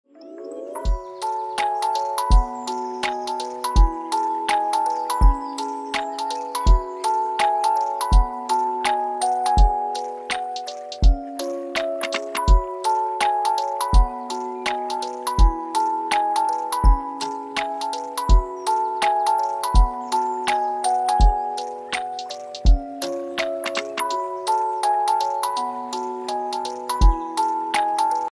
Nursery Artwork | Acrylic Paint Time Lapse | Owl Zebra Rabbit Painting
Nursery Artwork Painting. Time lapse.